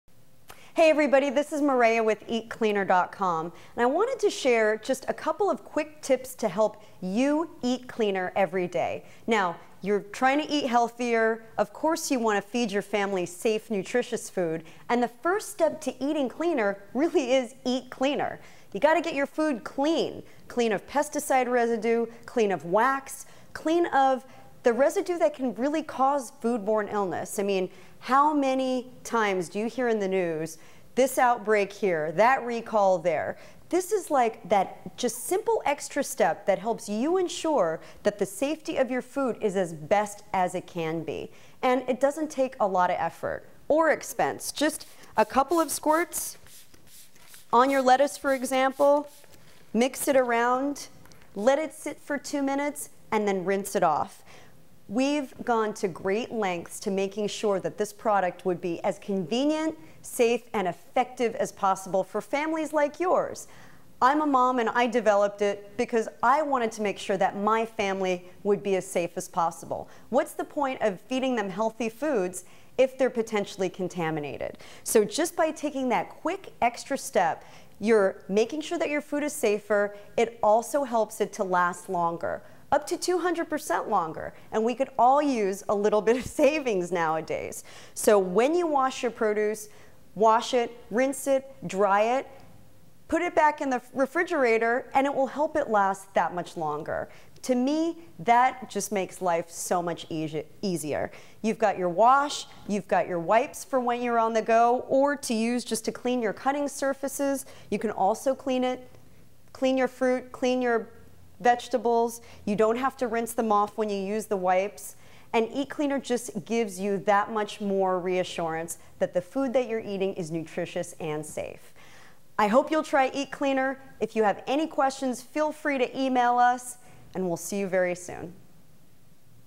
Safer, Cleaner Food How-To with Eat Cleaner
The first step to eating cleaner is Eat Cleaner, the only line of award-winning, all natural food wash + wipes lab proven to remove residue from fresh produce that water can't.  Also helps extend shelf life up to 200% longer, saving you money and your fruit + veggies from the trash!  Learn how to use these simple, safe products from Eat Cleaner inventor, Mareya Ibrahim.